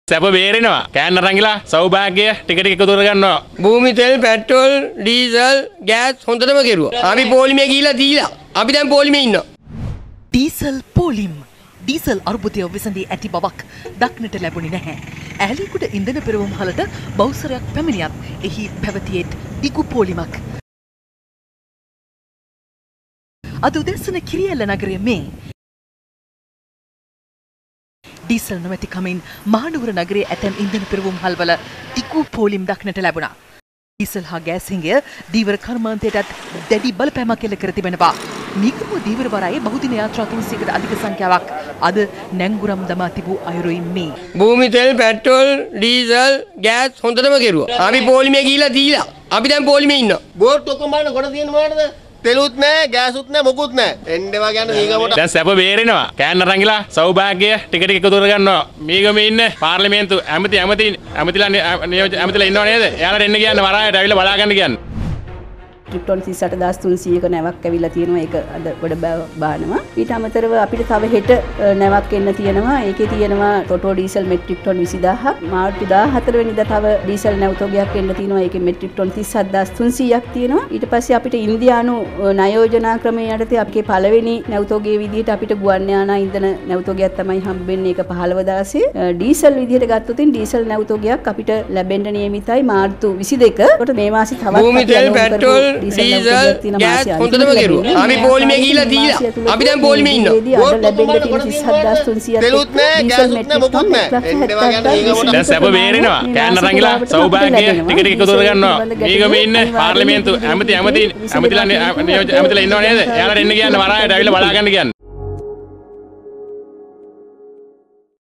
''දැන් සැප බේරෙනවා, කෑන්වලට සෞභාග්‍ය ටික ටික දාගන්නවා''
ඩීසල් ගැටලුව අනිවාර්යෙන් ඉදිරි දින කිහිපයේදී විසඳෙනවා  -බලශක්ති අමාත්‍යාංශය